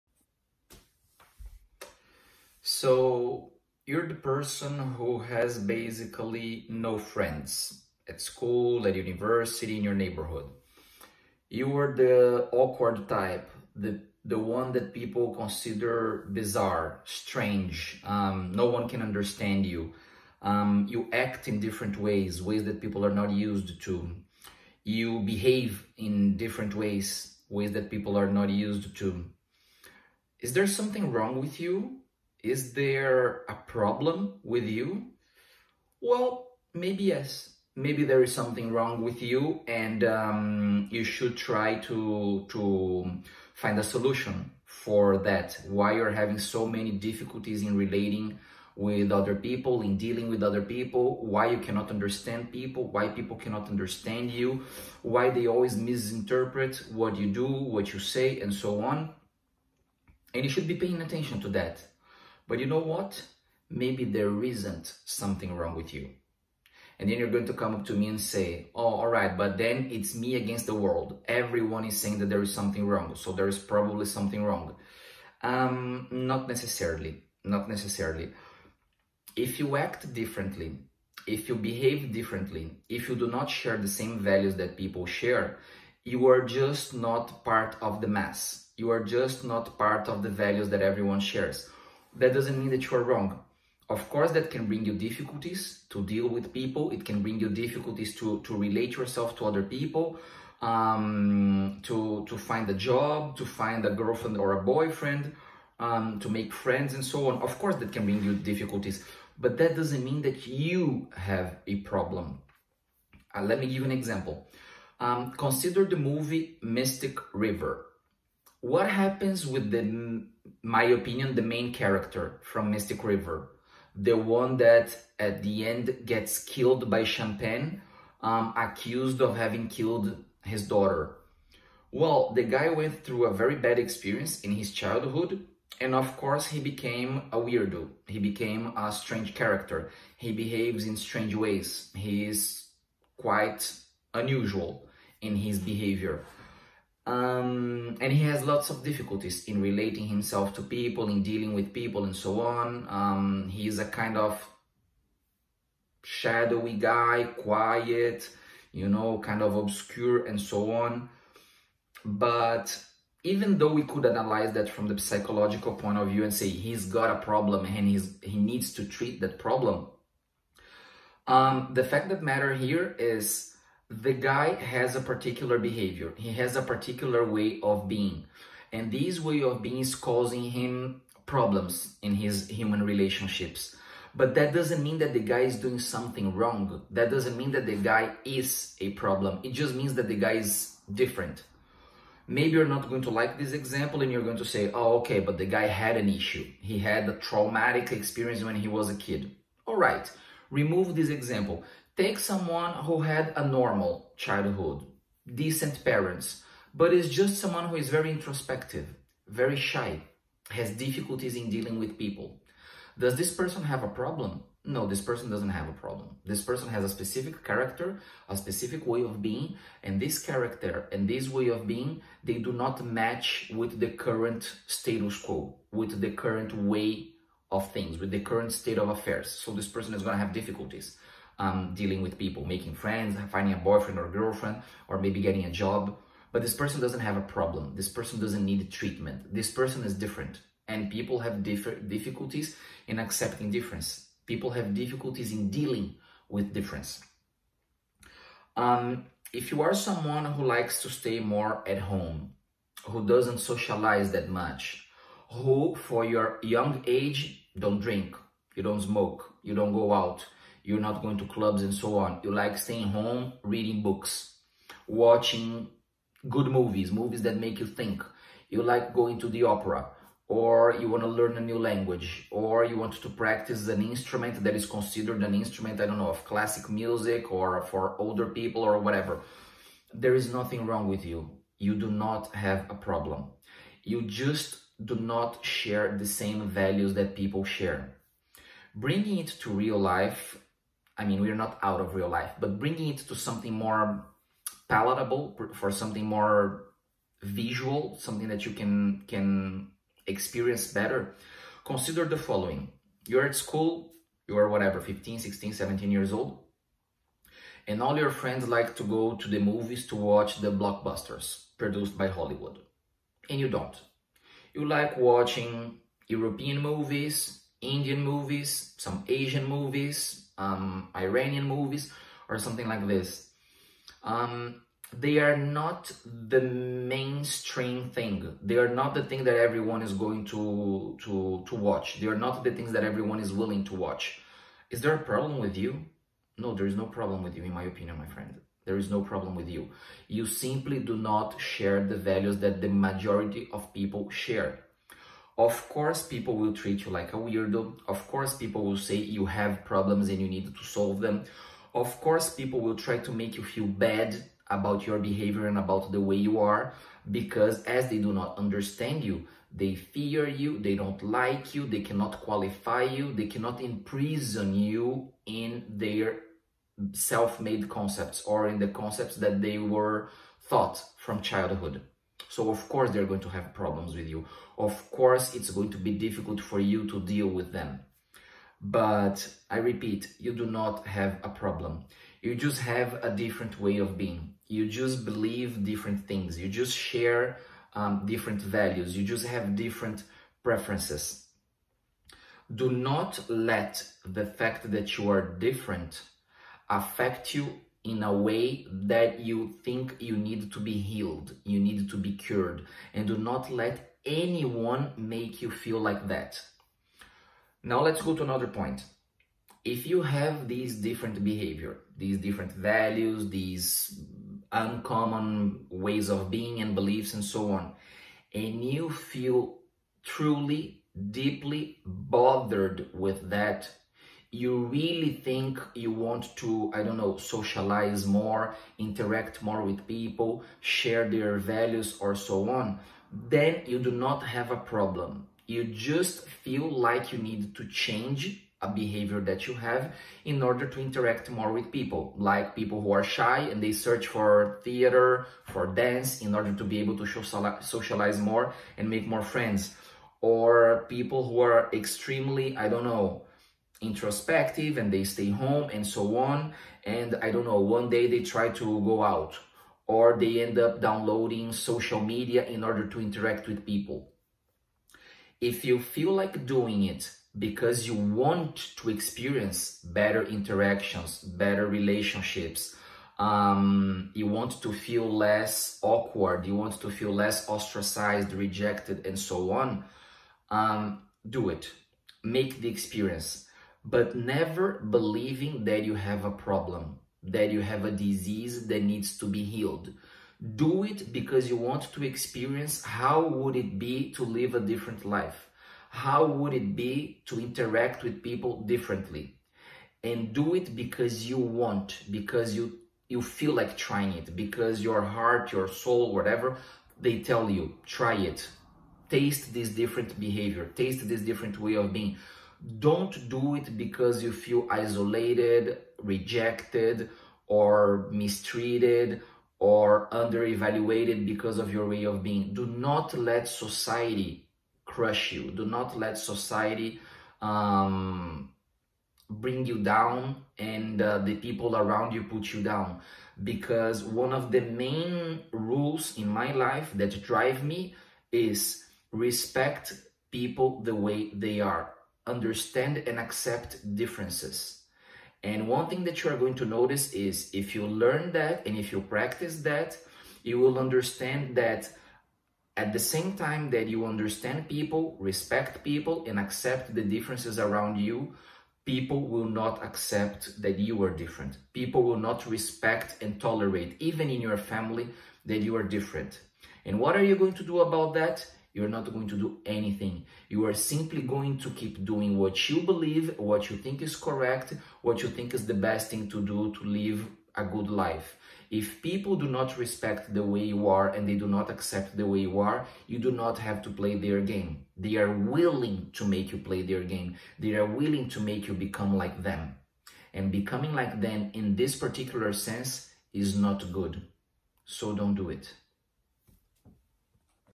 No friends? 1st Season (Dealing with people), 14th Episode
Are you the weirdo, the person who cannot make friends or that has difficulties with it? Are people always misinterpreting you? Do you have any parcel of responsibility for that? Let's find out together.